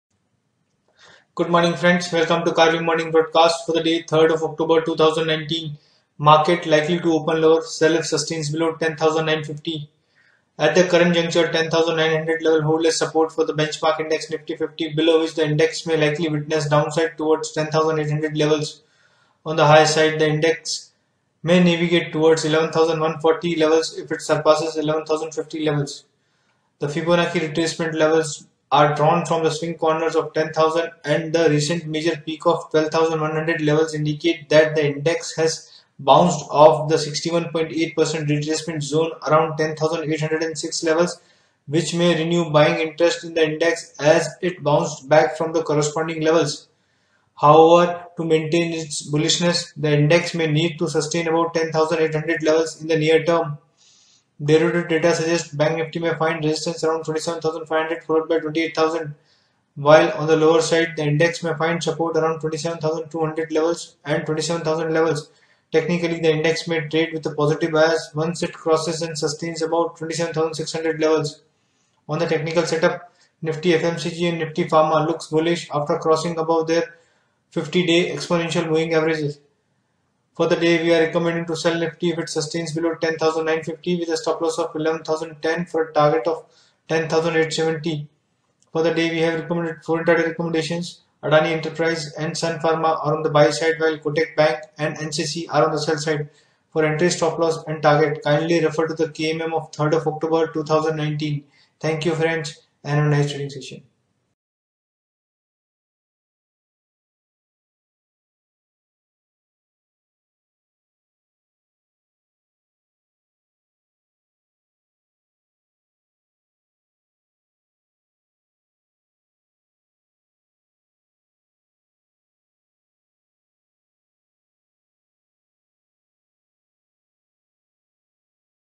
Market likely to open lower, sell if sustains below 10950.-Karvy Morning Moves (3-09-2019)
•At the current juncture, 10900 levels hold as support for the benchmark index Nifty50 below which the index may likely witness downside towards 10800 levels. On the higher side, the index may navigate towards 11140 levels if it surpasses 11050 levels.
•The Fibonacci retracement levels are drawn from the swing corners of 10000 and the recent major peak of 12100 levels indicate that the index has bounced off the 61.8% retracement zone around 10806 levels which may renew buying interest in the index as it bounced back from the corresponding levels. However, to maintain its bullishness, the index may need to sustain above 10800 levels in the near term.
•Derivative data suggests BankNifty may find resistance around 27500 followed by 28000, while on the lower side, the index may find support around 27200 levels and

27000 levels. Technically, the index may trade with a positive bias once it crosses and sustains above 27600 levels.
•On the technical setup, Nifty FMCG and Nifty Pharma look bullish after closing above their 50 days EMA.-Presented by Amit Samar (Research Analyst)